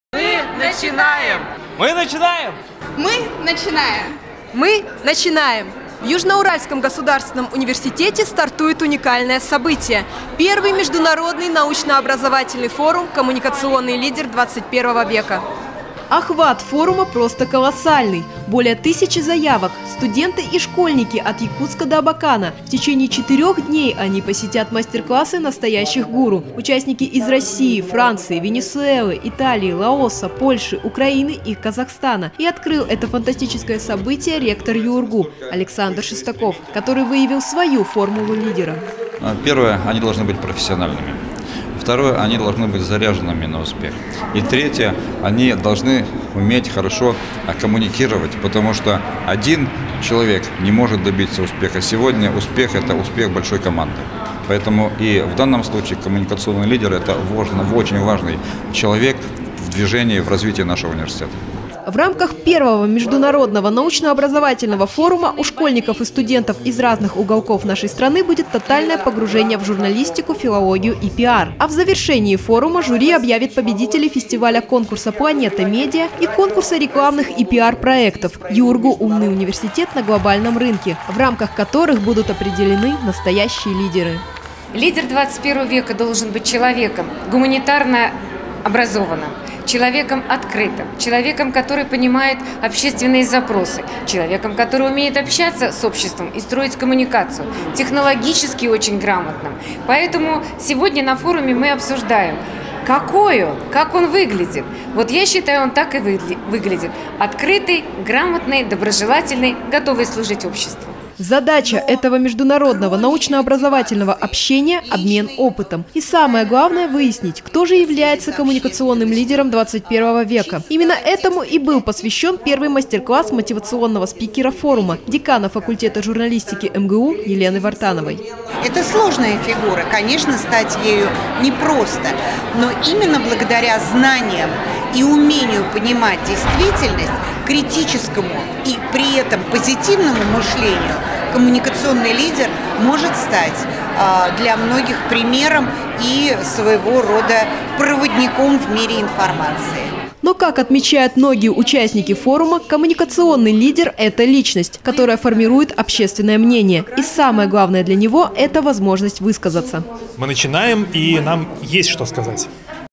Открытие I международного научно-образовательного форума «Коммуникационный лидер XXIвека»
В главном корпусе ЮУрГУ ажиотаж. Участники и почётные гости практически со всех уголков России готовы встретить главное событие марта.
В Южно-Уральском государственном университете стартует уникальное событие: Первый международный научно-образовательный форум«Коммуникационный лидер XXI века».
Охват форума просто колоссальный! Более 1000 заявок! Студенты и школьники! От Якутска до Абакана.В течение четырёх дней они посетят мастер-классы настоящих гуру. Участники из России Франции, Венесуэлы, Италии, Лаоса, Польши, Украины и Казахстана. И открыл это фантастическое событие ректор ЮУрГУ Александр Шестаков, который вывел свою формулу лидера.
В рамках I международного научно-образовательного форума у школьников и студентов из разных уголков нашей страны будет тотальное погружение в журналистику, филологию и PR. А в завершении форума жюри объявит победителей фестиваля-конкурса «Планета Медиа» и конкурса рекламных и PR-проектов «ЮУрГУ — умный университет на глобальном рынке», в рамках которых будут определены настоящие лидеры.
Задача этого международного научно-образовательного общения —обмен опытом. И самое главное выяснить, кто же является коммуникационным лидером XXIвека. Именно этому и был посвящён первый мастер-класс мотивационного спикера форума, декана факультета журналистики МГУ Елены Вартановой.
Но как отмечают многие участники форума, коммуникационный лидер — это личность, которая формирует общественное мнение. И самое главное для него — это возможность высказаться!